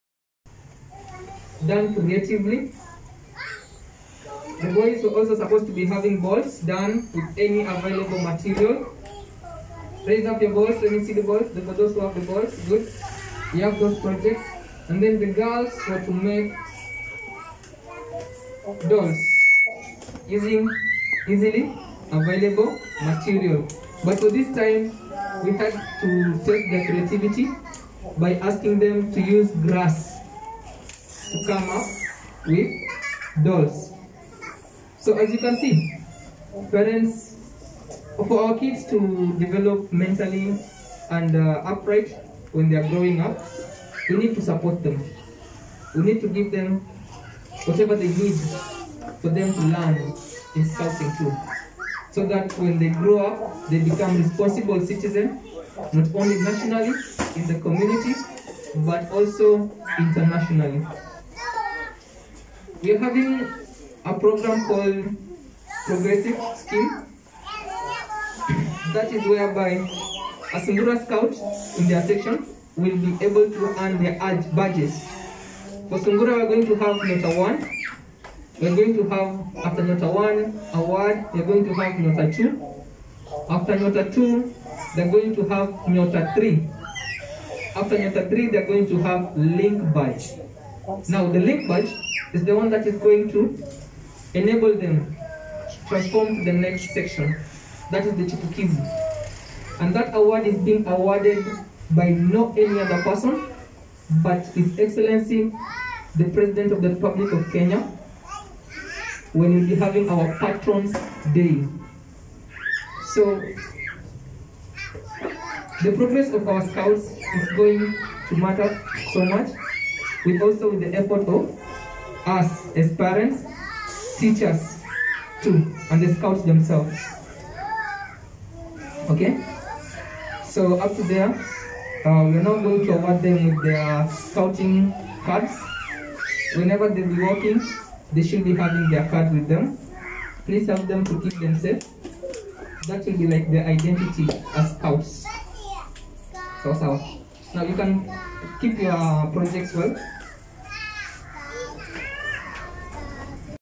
SHIKUKU TV:YELLOW ROSE SEA SCOUT CBC PRACTICALS. MAKING ITEMS USING THE AVAILABLE RESOURCES POSSIBLE
SHELDON KIPLAGAT - SCOUT WOOD BADGE LEADER AND MADAM LOLIAN TAKING CHARGE